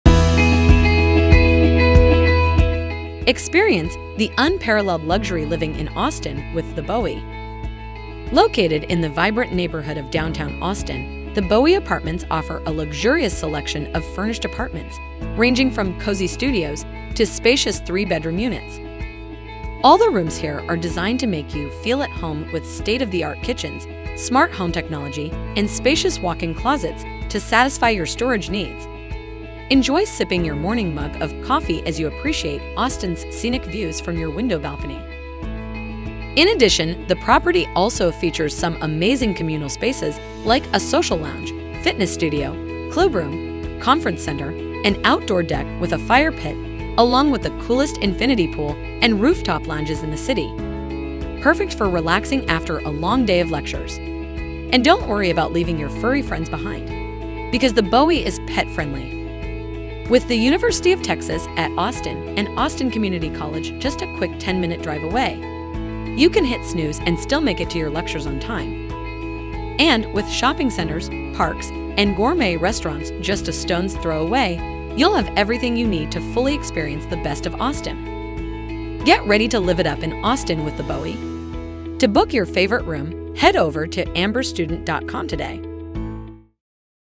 Property Tour | The Bowie Apartments | Student Accommodation in Austin | USA | amber
The Bowie Apartments is one of the most popular and lavish student housing in Austin, providing students with a truly exceptional experience. Present near Colorado River, you will find a wide range of luxurious studios, 1 and 2 bed baths apartments. The University of Texas is at an 11-minute distance and Austin Community College is also nearby to the property that makes it a great student housing in Austin.

*Features*
Each of the room here is designed keeping in mind the student's needs and comes with a comfortable bed, wardrobe, study desk and plenty of storage for all your belongings. The property offers some of the most incredible amenities like a state-of-the-art fitness center equipped by Precor on rooftop, and resort style rooftop infinity-pool. You can also find an outdoor deck with fire pit, grilling area, and numerous seating areas on the 31st floor. There is a clubroom and a conference room with multiple television facilities. Furthermore, utilities like water, Wi-fi, gas and electricity are all included in the bill.

The property is surrounded by some of the city's finest restaurants and bars like Super Burrito Restaurant and the Tiniest Bar In Texas where students can hangout during the weekend. You can also find popular supermarkets like Trader's Joe and Whole Foods Market just within walking distance from the property. Many cities' retail and leisure centres are located near this and another popular road known as Bowie Street.

*Commute*
You can hop onto a train from Austin Central Train Station or catch a bus from 5th/Bowie Bus Stop which is only a short walk away from the property and K And K Cab Austin Taxi Service is also present nearby the student housing. The peaceful surroundings and lively study room helps you in your studies. In all, The Bowie student housing provides the best housing experience, to keep the right balance of study and social life.